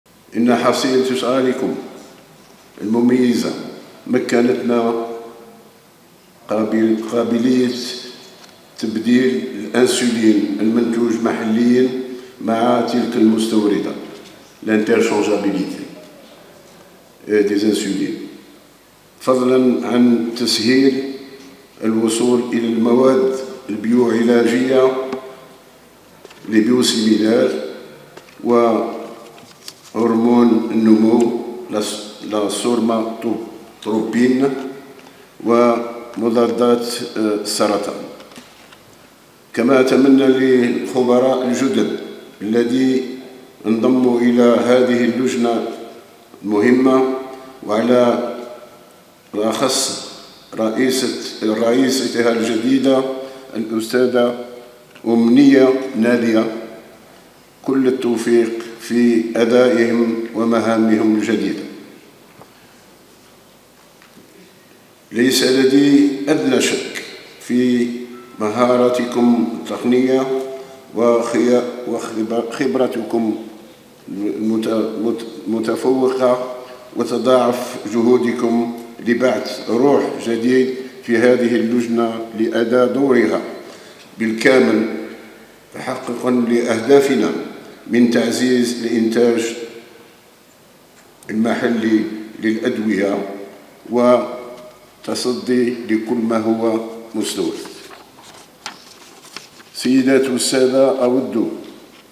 كلام قوي من وزير الصناعة والإنتاج الصيدلاني لأعضاء لجنة الخبراء العياديين الجدد خلال تنصيبهم !!
شاهد ما قاله وزير الصناعة والإنتاج الصيدلاني لأعضاء لجنة الخبراء العياديين الجدد خلال تنصيبهم لدى الوكالة الوطنية للمواد الصيدلانية